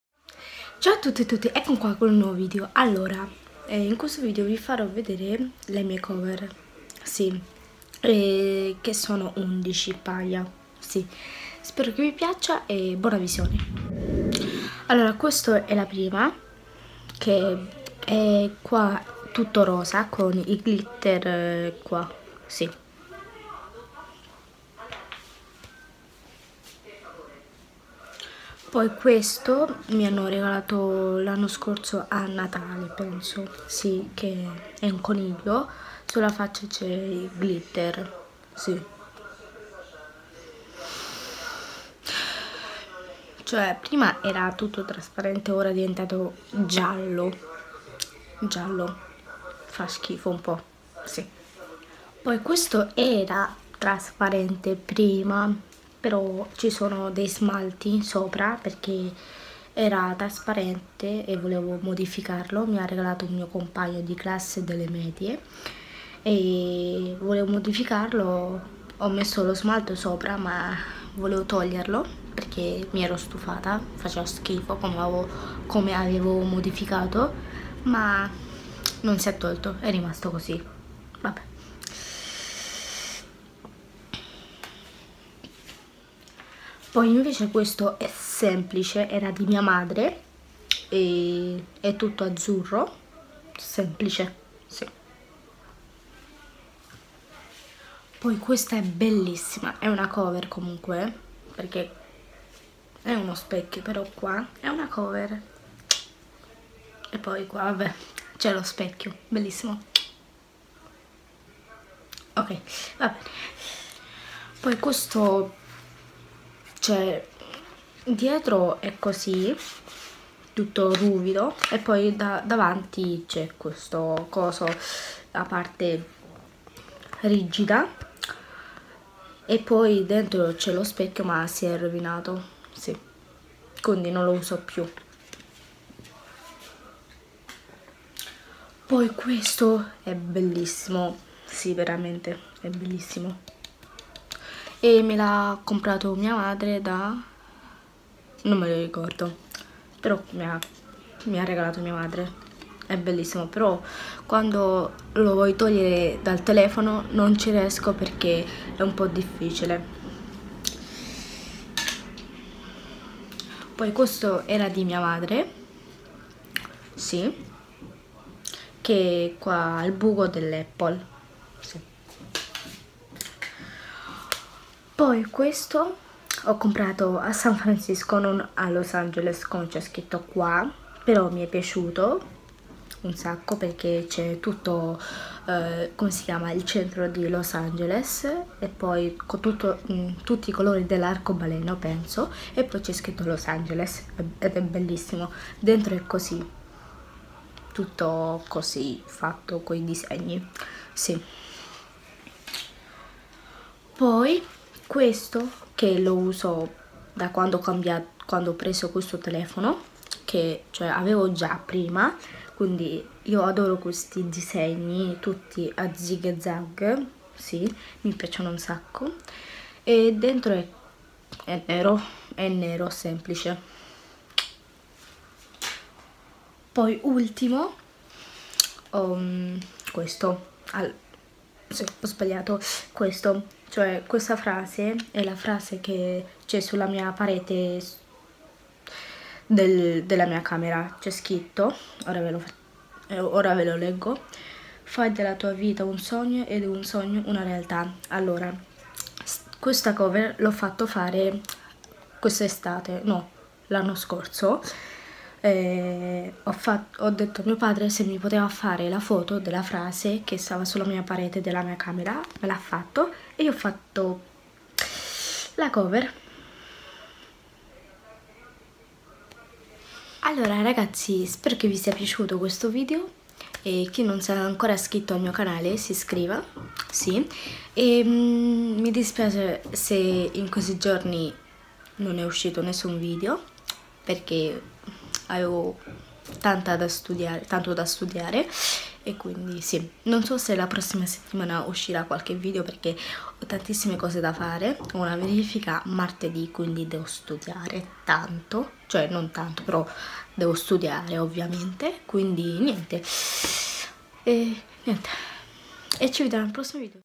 COVER PER IPHONE... NATALEINSIEME Golapi bronzetti
Ciao tutti a tutti in questo video vi farò vedere le mie cover per iPhone
spero che vi piaccia BUONA VISIONE...

Nome:golapi
Età:16

Informazioni
Registro con:Videocamera sony
Edito.Imovie